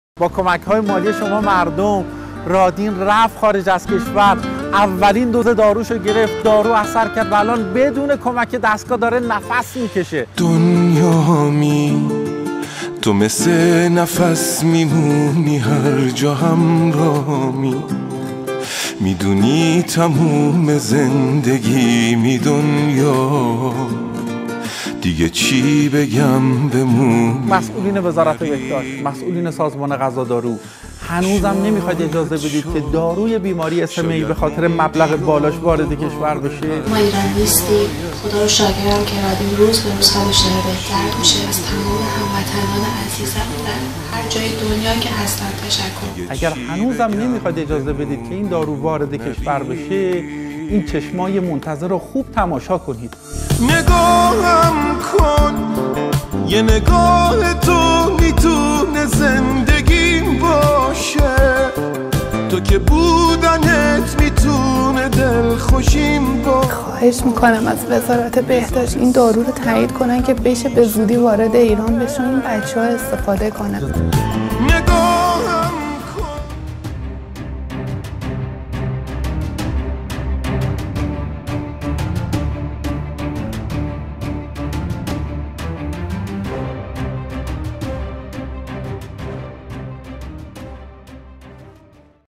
مردم ایران سه میلیارد پول جمع کردن این بچه رو فرستادن خارج
وقتی نذاشتن این دارو وارد کشور بشه ، مردم ایران هم سه میلیارد پول جمع کردن این بچه رو فرستادن خارج ...
حالا نتیجه رو ببینید

برای مشاهده جدیدترین ویدیوهای روز کانال ما را دنبال کنید